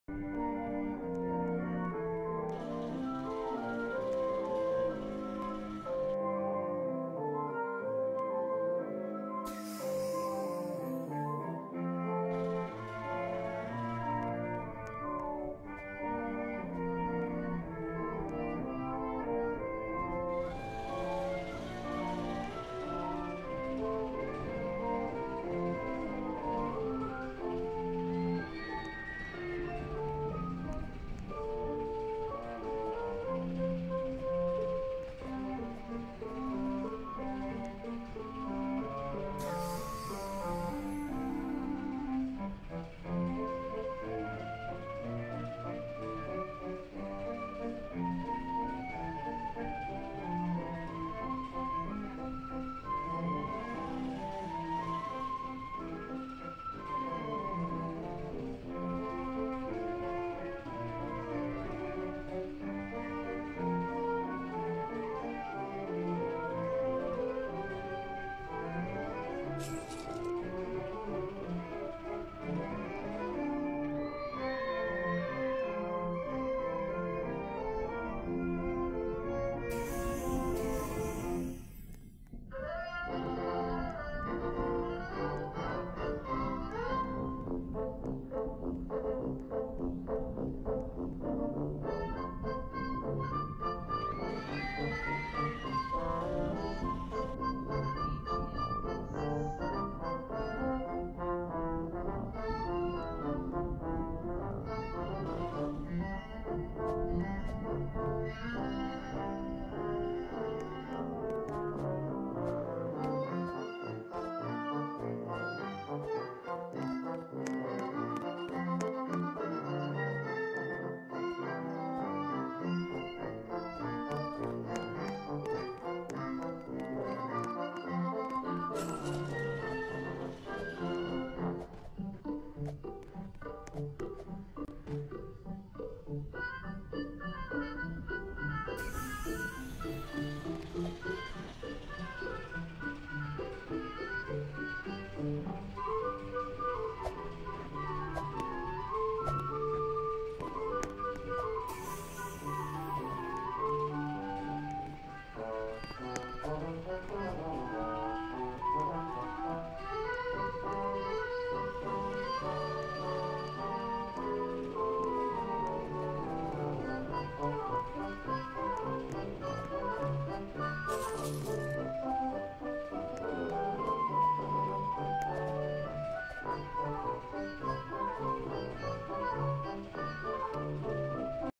Grand Opening Of Peter Pan's Flight
So silly me forgot to take you guys onto the ride in this video. So I have another video uploading after this one that features the ride. If I knew how to edit I'd make it all one video but idk how sadly.